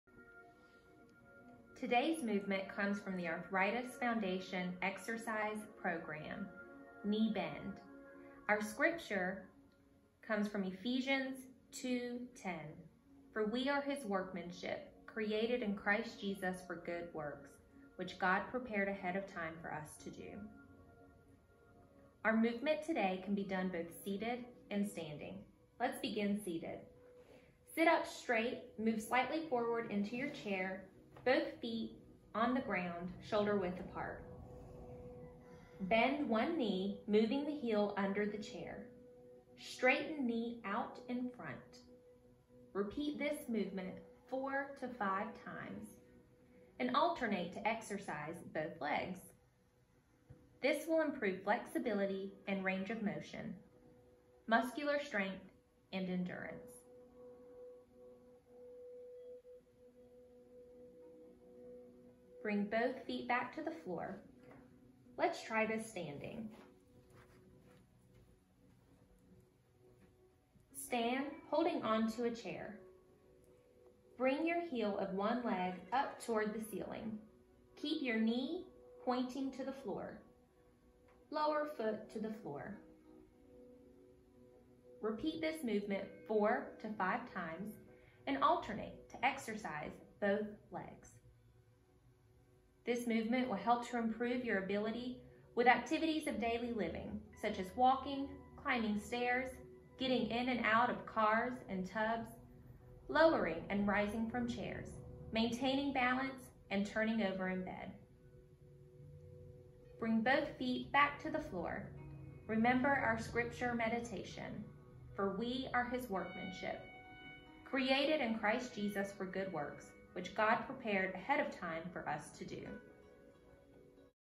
Knee Bend
Welcome to Sixty Second Soul Stretch! Team members from NCBAM offer you movement, mindfulness, and God’s word to calm your mind, relax your body, and stretch your soul. Take a moment and enjoy "Knee Bend" from the Arthritis Foundation Exercise Program.